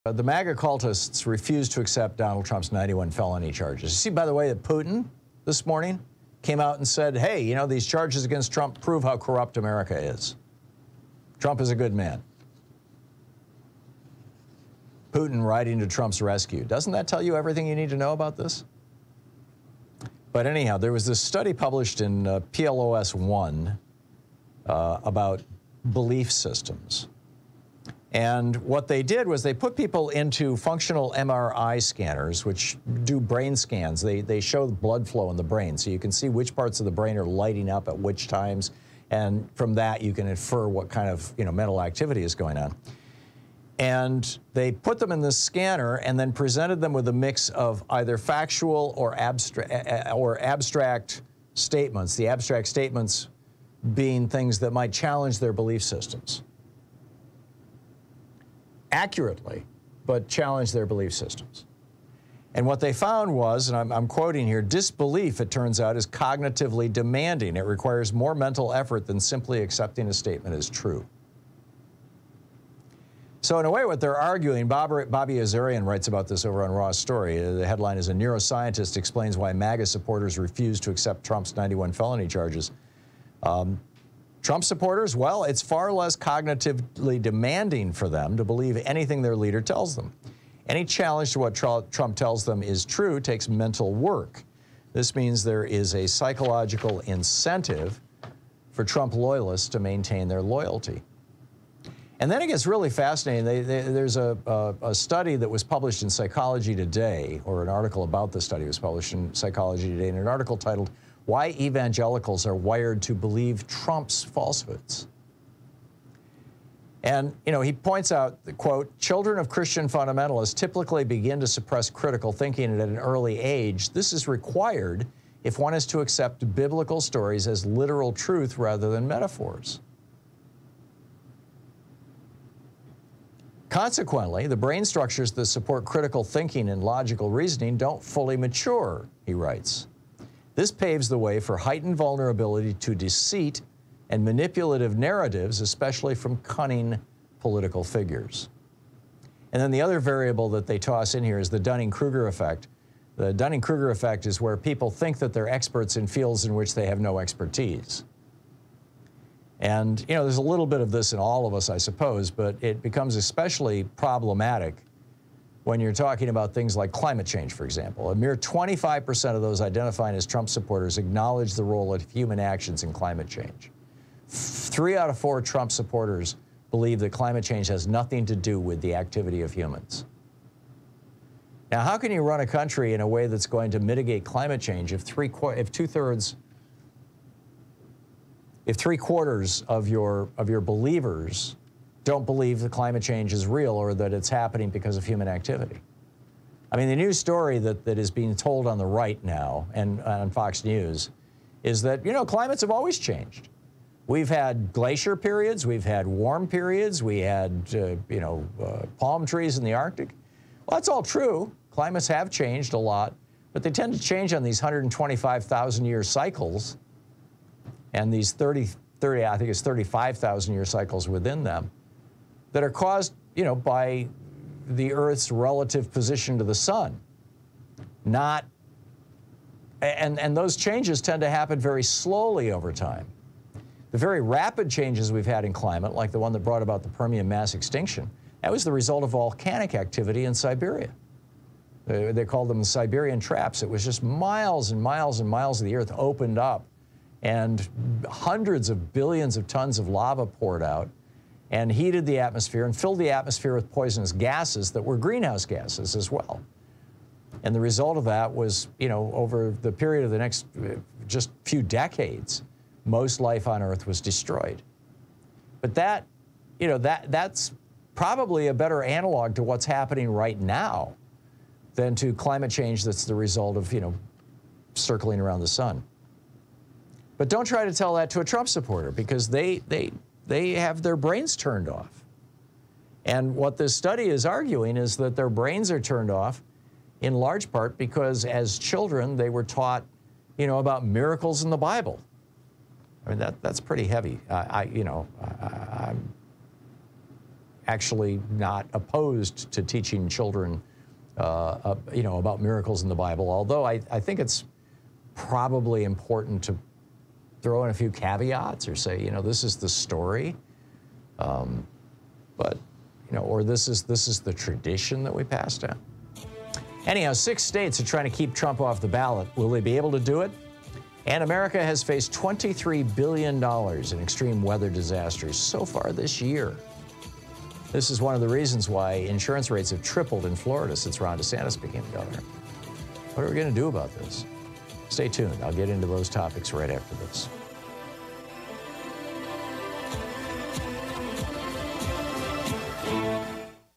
Science Explains Why Republicans Can’t Accept Trump’s Guilt
Scientists discovered a fascinating reason why Republicans can’t accept criticism of Donald Trump. Thom explains.